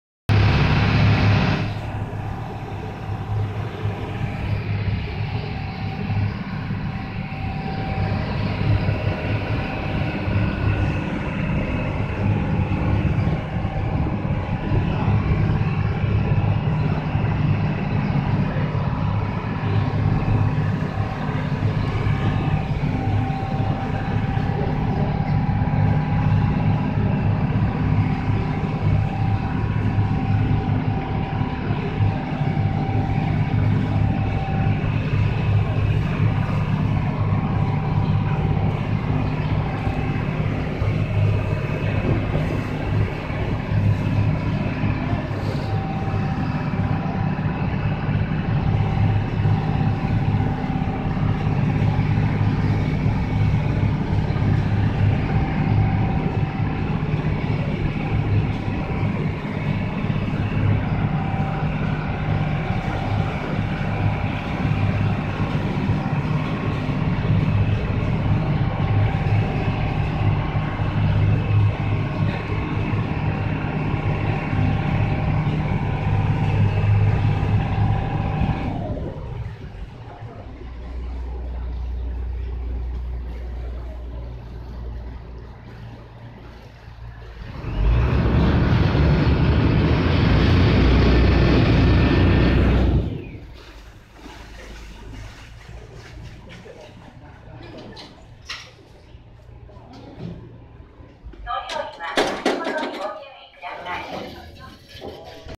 天保山渡し舟 2014.8
此花区側から天保山への２分ほどの経路。
主に進行方向右側を撮影。
ＵＳＪの隣の駅から少し歩くと乗れます。
大雨の後で海の色が砂色っぽかったかな。

※撮影機種：ARROWS F-06E スマホです。